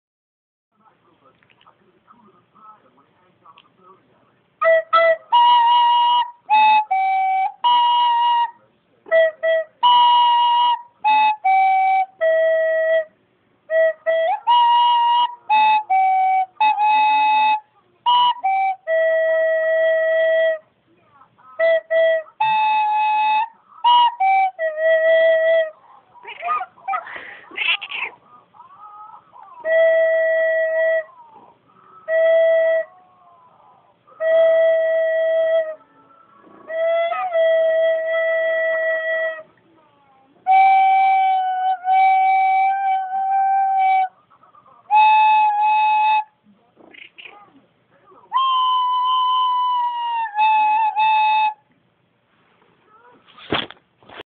Boo the cat & the recorder.
Boo decided practicing the recorder is dumb.